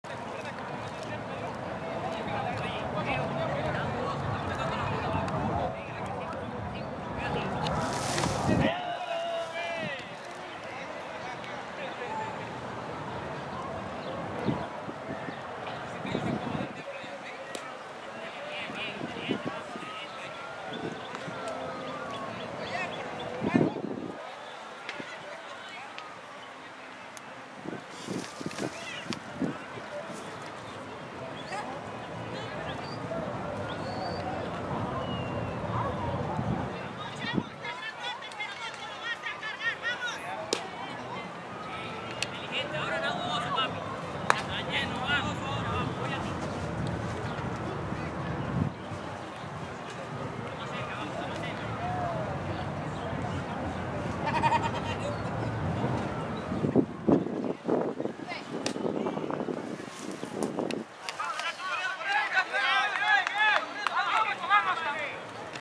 Dover 15 (vs) Cachorros 0 - En 73.2016
Los chicos de New Jersey noquearon a sus rivales en la quinta entrada.